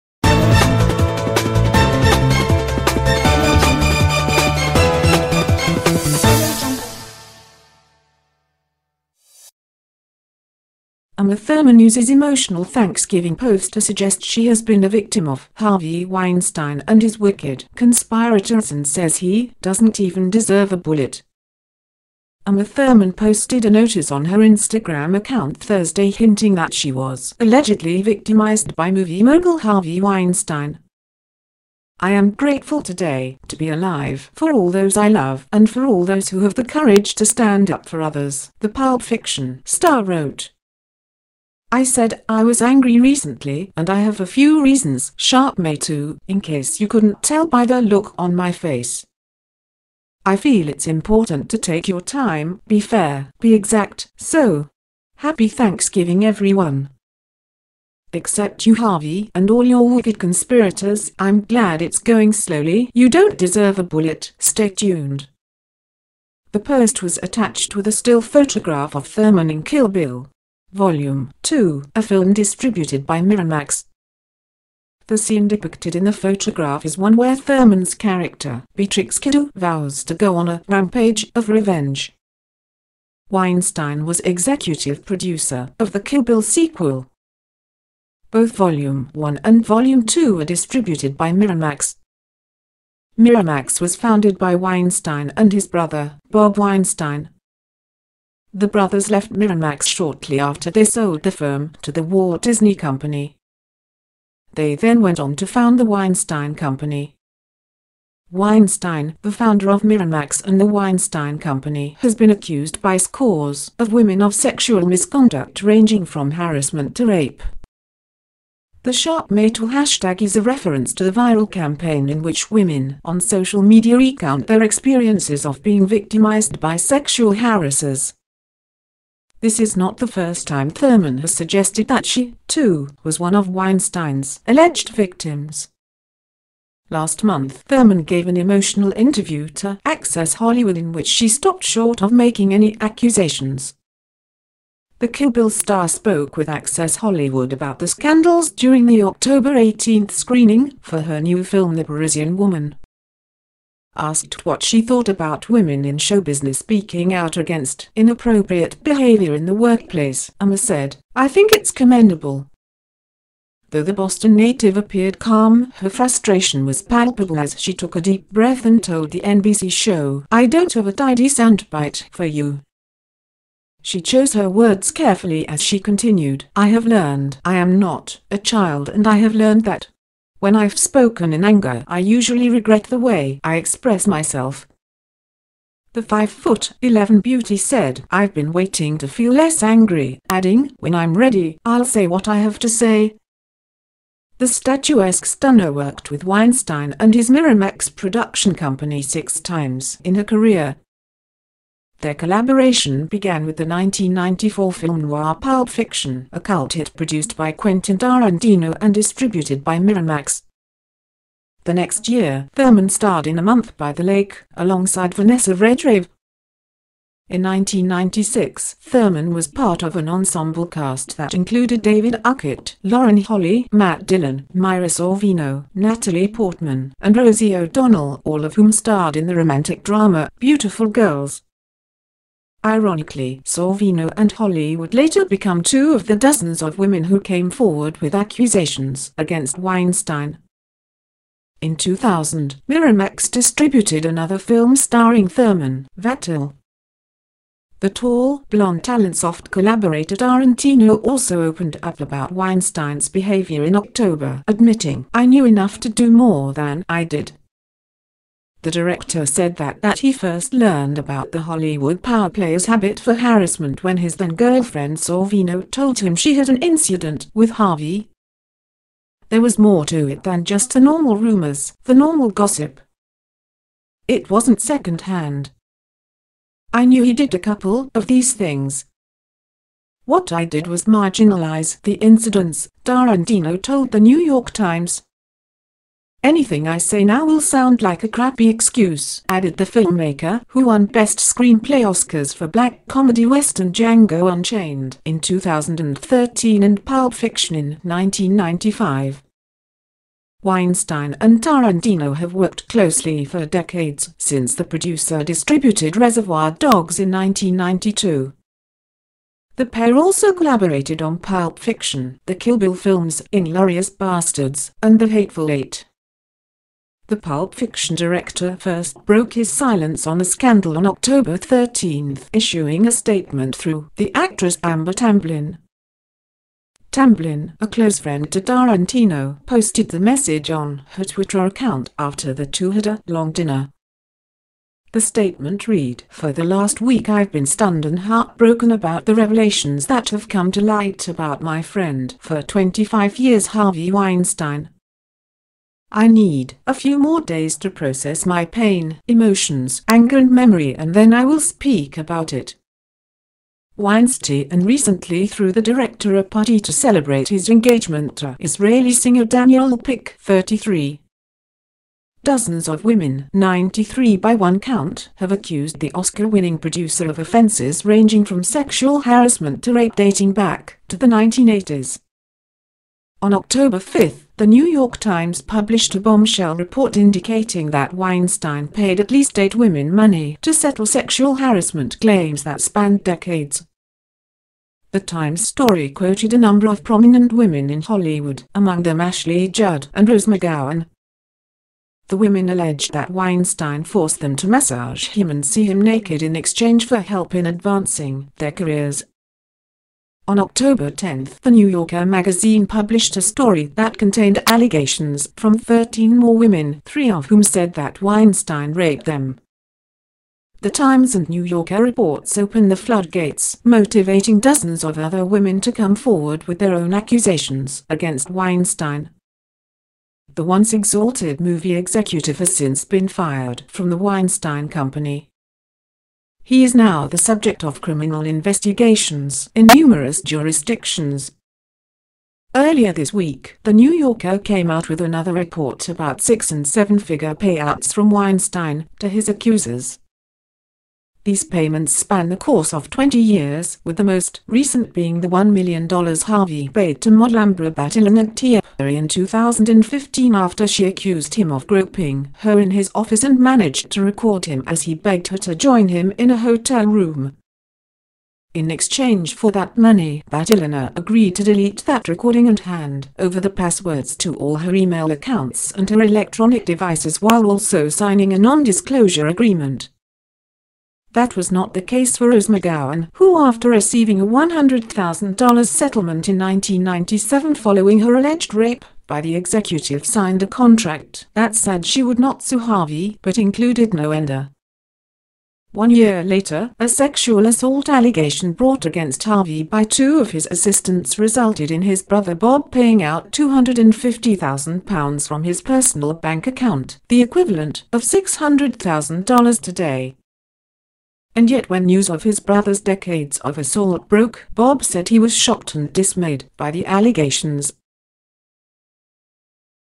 Uma Thurman uses emotional Thanksgiving post to suggest she has been a victim of 'Harvey Weinstein'
Uma Thurman posted a notice on her Instagram account Thursday hinting that she was allegedly victimized by movie mogul Harvey Weinstein.
‘I am grateful today, to be alive, for all those I love, and for all those who have the courage to stand up for others,’ the Pulp Fiction star wrote.